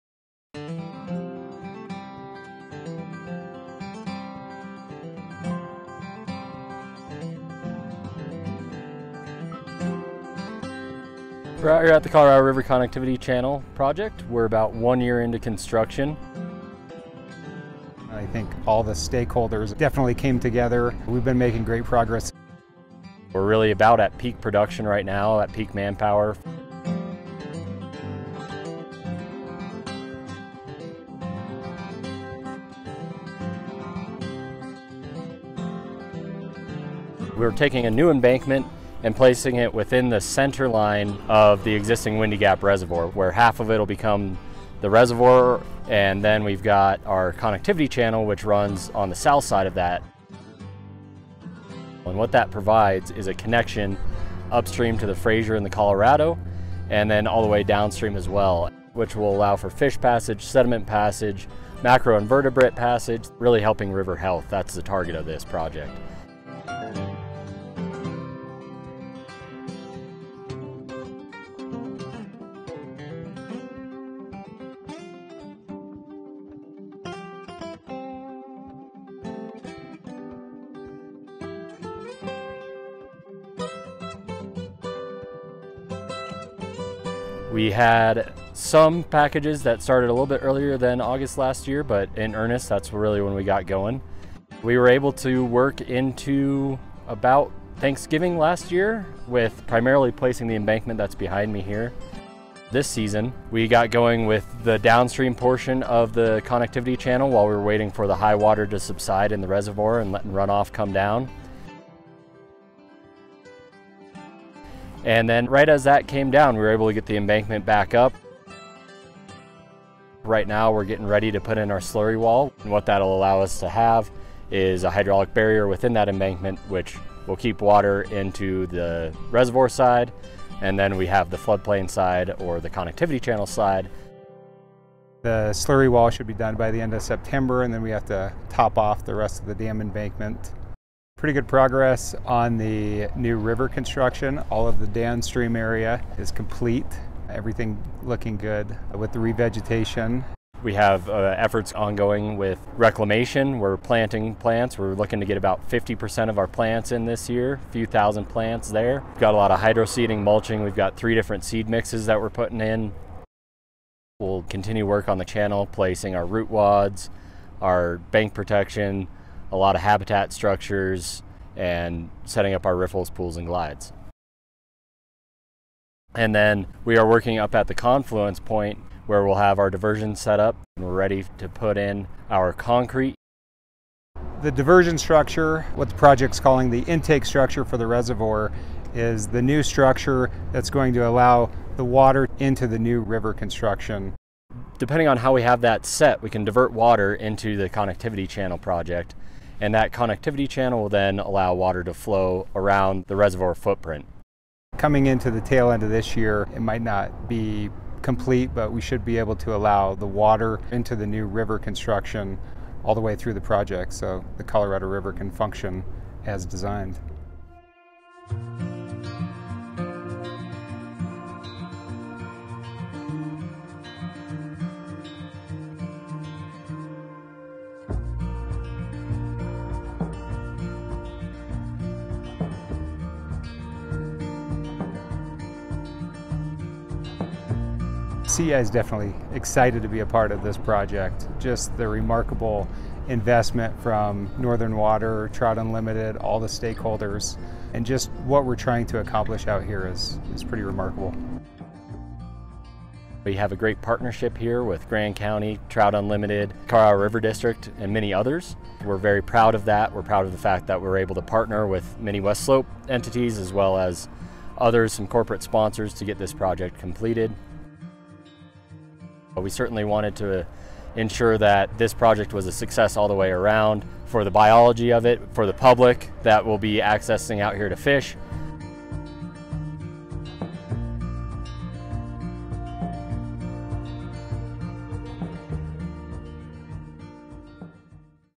Colorado River Connectivity Channel 1-Year Construction Update
Crews at the Colorado River Connectivity Channel (CRCC) site are hitting their stride one year into construction. A flurry of activity has been underway since the Aug. 23, 2022, groundbreaking, and in this new video, project managers from Northern Water and CRCC general contractor, CEI, provide updates on what all has taken place this past year, and also what’s ahead for the project.

Once completed, the new channel will meet a longtime goal of connecting two stretches of the Colorado River that were bisected by the construction of Windy Gap Reservoir and Dam in the mid-1980s. The dam will continue to provide a diversion point on the Colorado River for the Windy Gap Project during the high flows of spring and early summer. Otherwise, water will flow through the new channel, and that connection will make it possible for fish and other wildlife to move freely upstream and downstream around the reservoir and dam.

The CRCC is part of a $90 million package of environmental measures associated with construction of Chimney Hollow Reservoir, which is where Windy Gap Project water will be stored once it’s built.